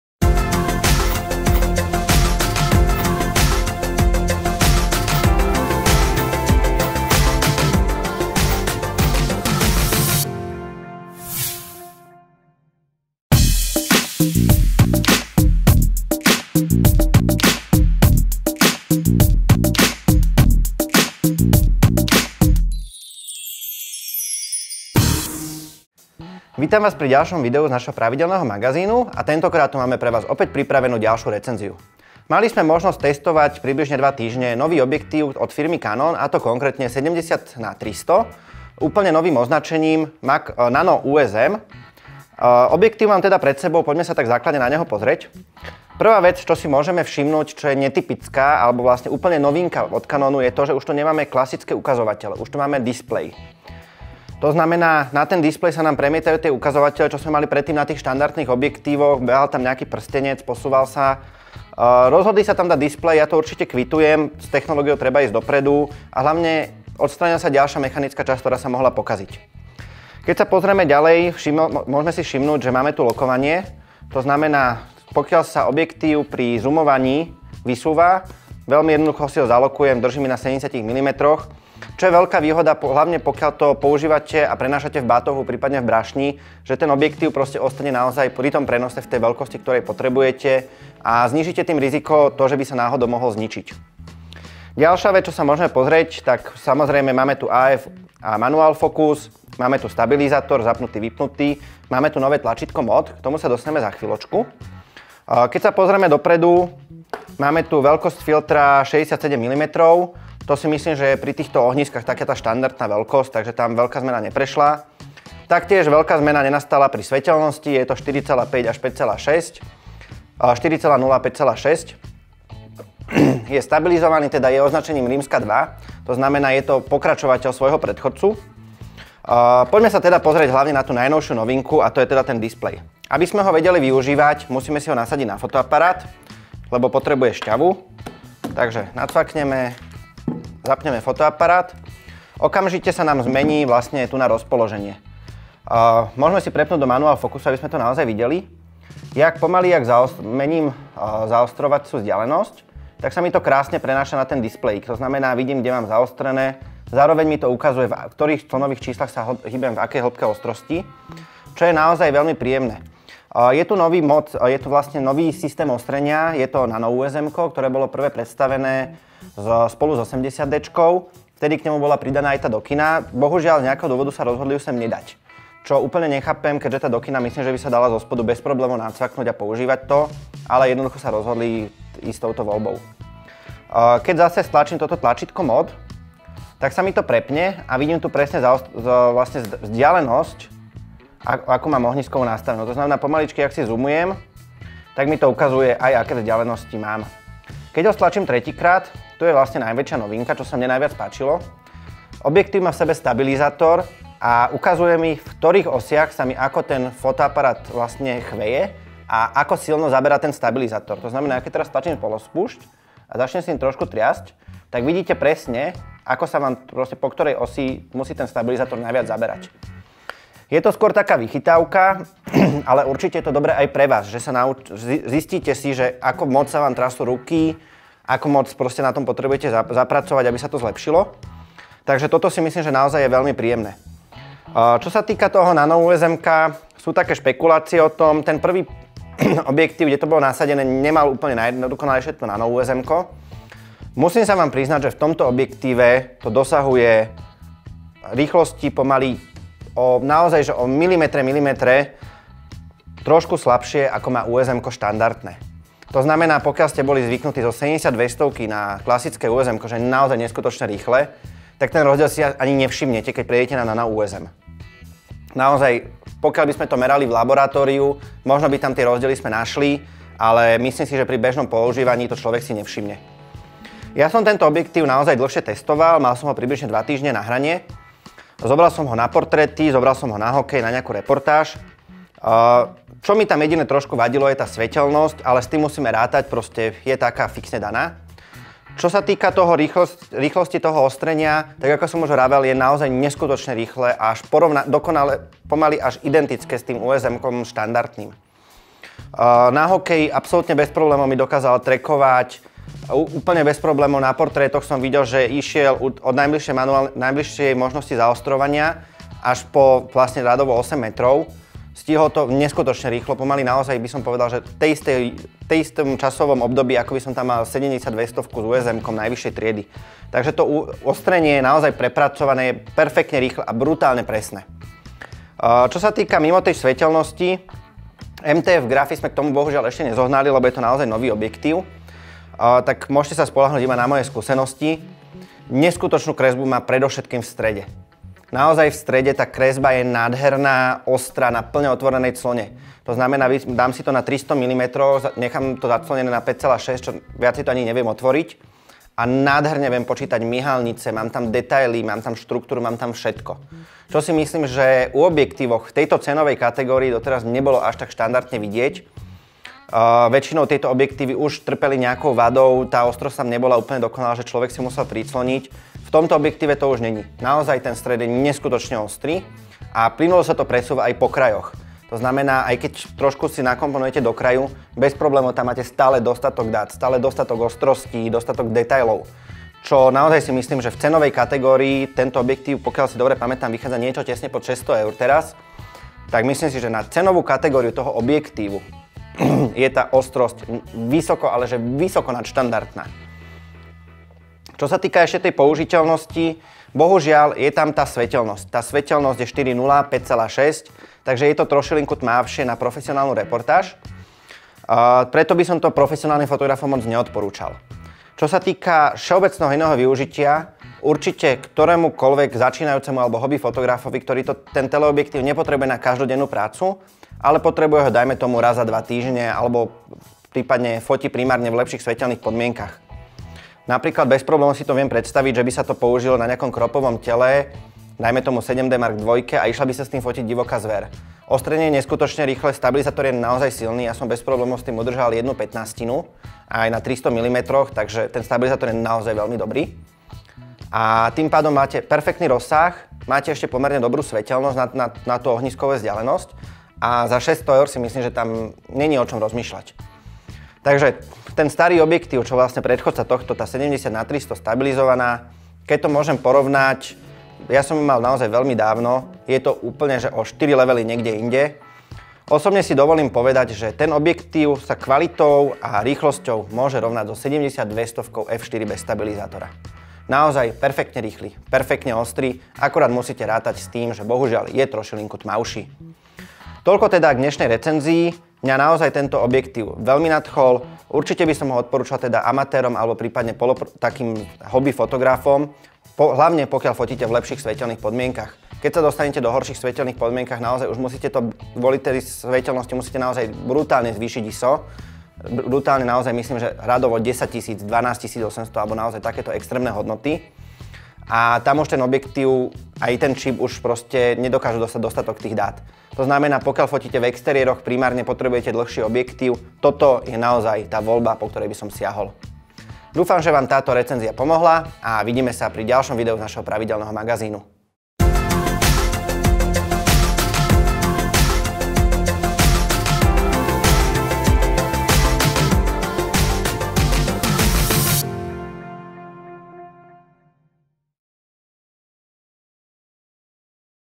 Canon EF 70-300mm F/4-5,6 IS II USM
Výkonný, no pritom cenovo dostupný model EF 70-300 mm f/4-5,6 IS II USM bude atraktívny pre fotografov prírodných krás a športov, ktorí majú záujem o dosahovanie ostrejších snímok pri fotení z ruky. Detail produktu